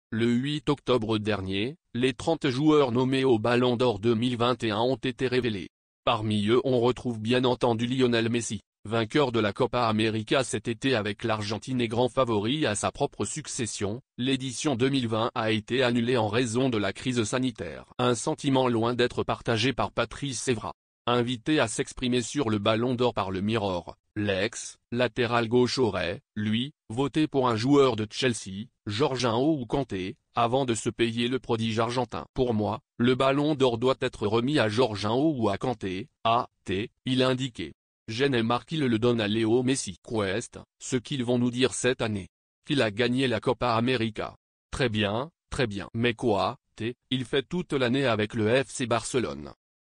Evra détruit Messi
Le 8 octobre dernier, les 30 joueurs nommés au Ballon d’Or 2021 ont été révélés. Parmi eux on retrouve bien entendu Lionel Messi, vainqueur de la Copa America cet été avec l’Argentine et grand favori à sa propre succession – l’édition 2020 a été annulée en raison de la crise sanitaire. Un sentiment loin d’être partagé par Patrice Evra. Invité à s’exprimer sur le Ballon d’Or par le Mirror, l’ex-latéral gauche aurait, lui, voté pour un joueur de Chelsea, Jorginho ou Kanté, avant de se payer le prodige argentin. « Pour moi, le Ballon d’Or doit être remis à Jorginho ou à  Kanté, a-t-il indiqué. J’en ai marre qu’ils le donnent à Leo Messi. Qu’est-ce qu’ils vont nous dire cette année ? Qu’il a gagné la Copa America ? Très bien, très bien. Mais qu’a-t-il fait toute l’année avec le FC Barcelone ? »